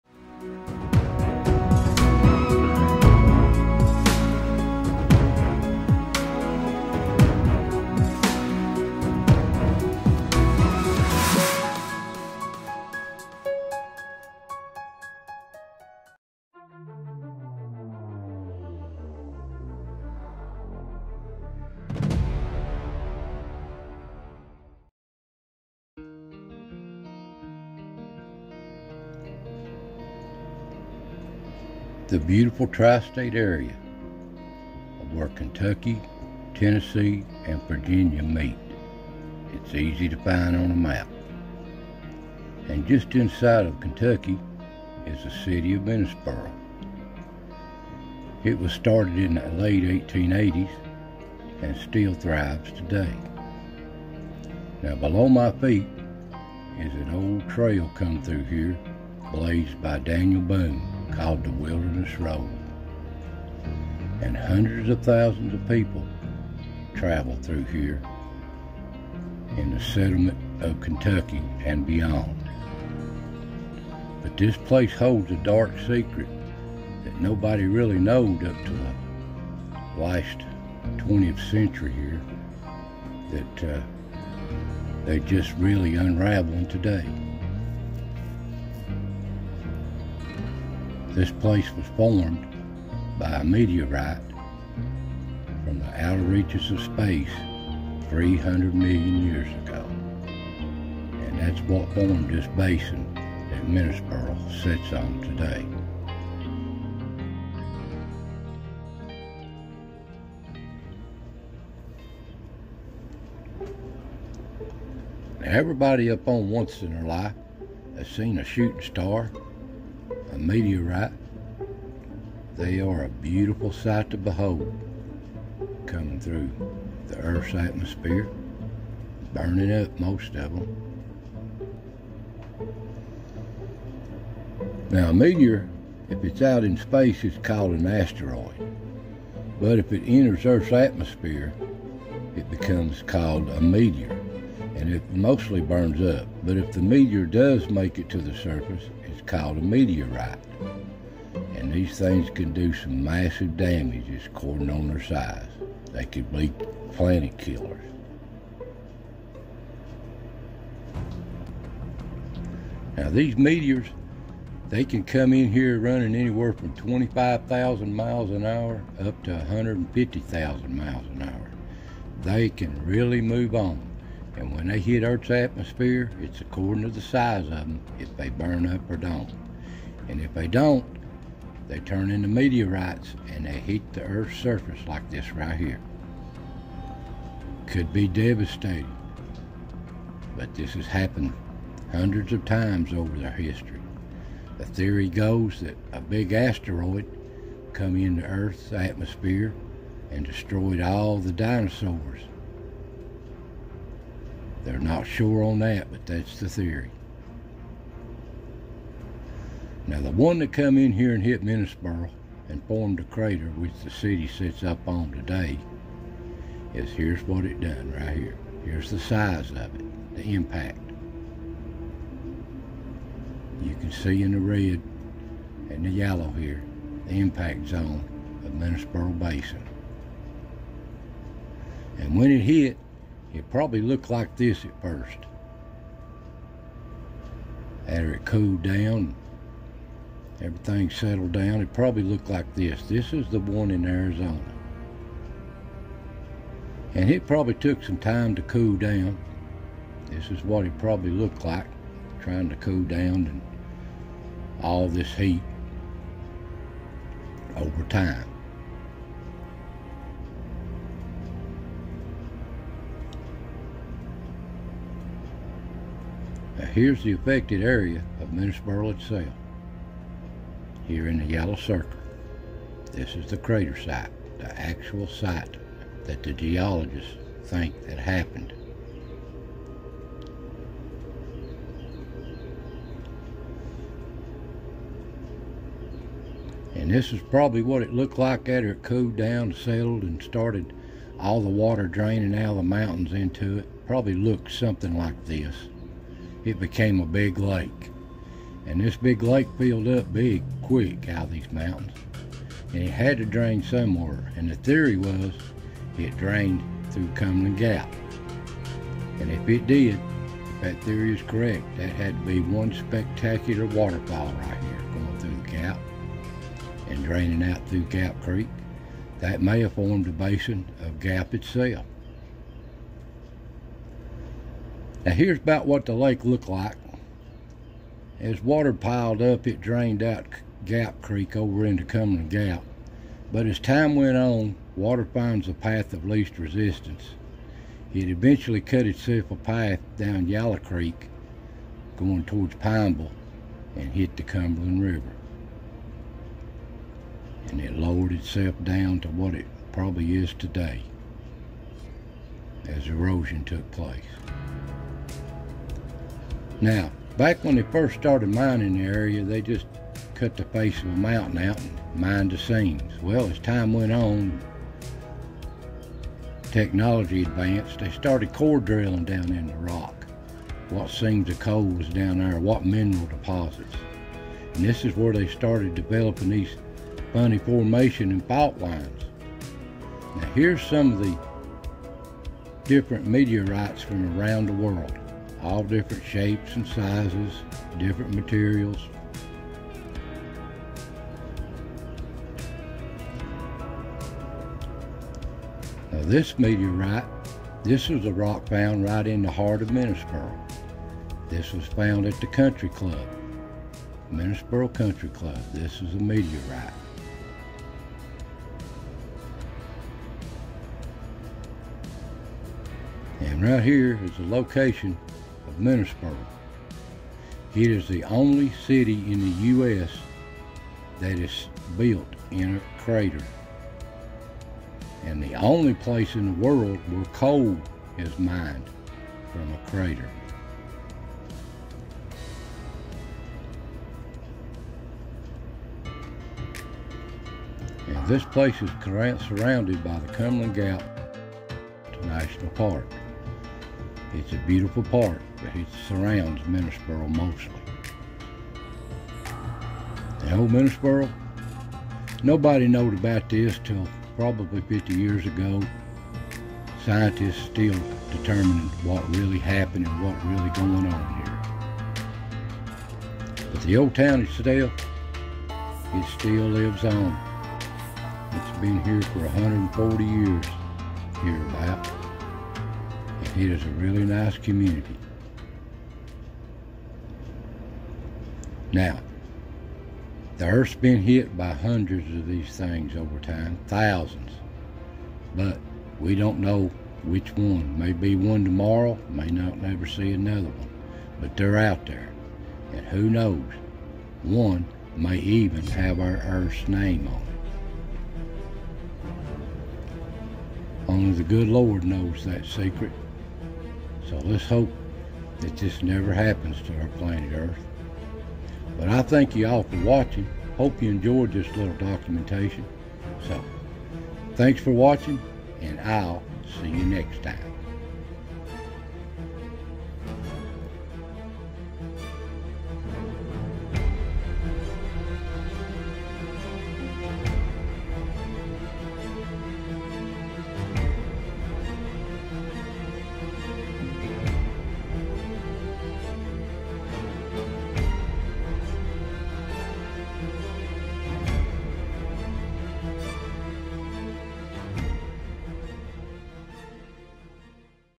Middlesboro, Kentucky, The City built in a Meteor Crater.
Hard to believe that close to 300 million years ago a piece of rock from outer space formed the Crater that the city of Middlesboro sets on today. It also helped shape the landscape of this mountainous area.